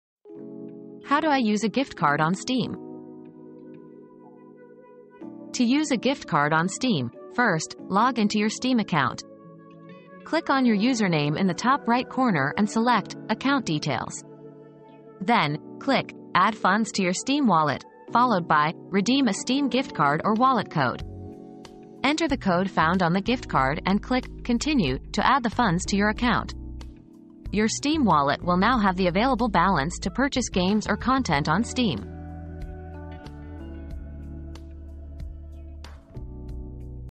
How do I use a gift card on Steam?
Steam Gift Card Redemption Guide • Steam Gift Card • Learn how to easily redeem a Steam gift card in just a few simple steps. Add funds to your Steam Wallet and start purchasing games and content today!